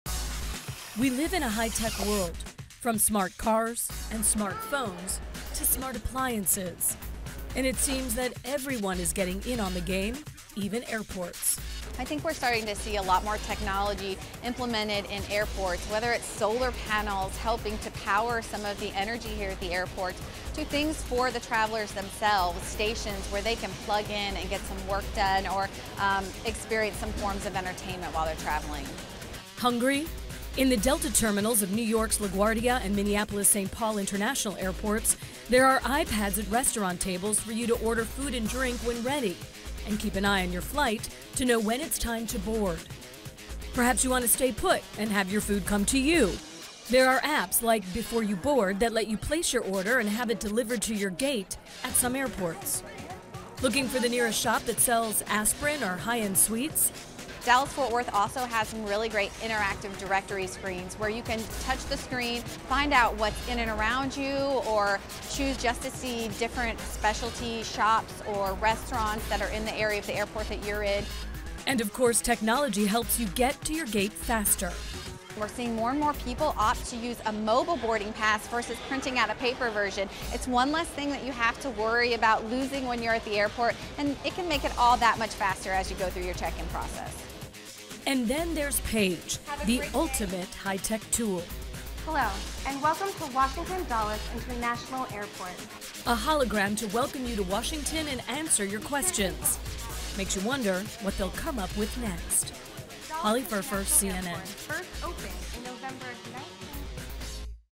How airports are embracing technology for the passenger
We highlight some of the technology airports are providing for the frequent traveler.